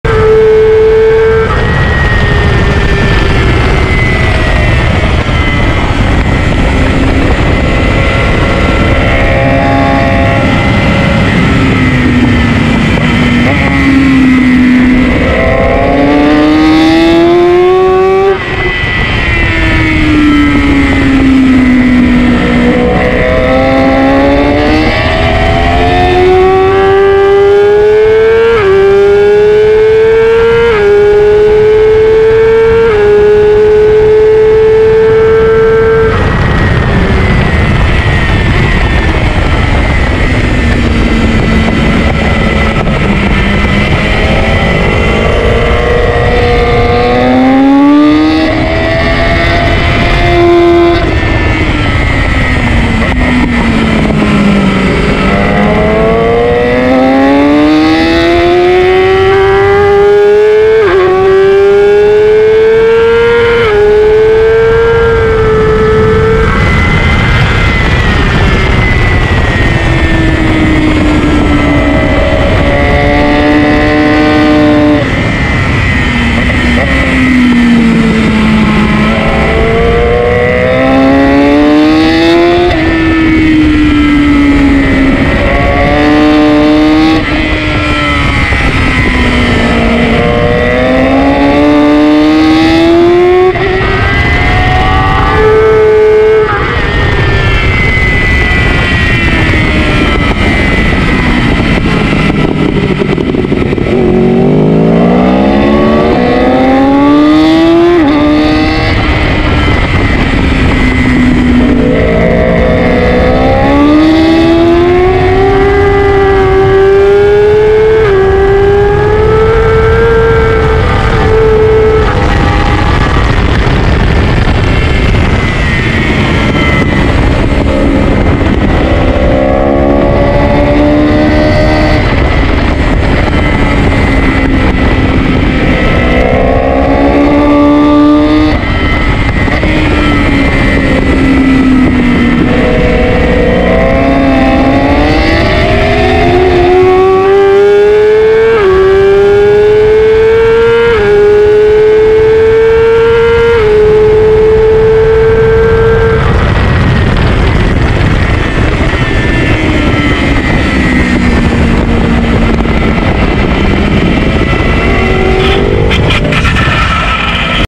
NCM Trackday Winner 2:00 Lap with crash
I was looking to get my sub 2:00 lap at NCM and apparently asked too much of my tires during a morning session with not ideal track. Overcast after the misting stopped.  I fixed it up and kept riding but had lost my corner confidence.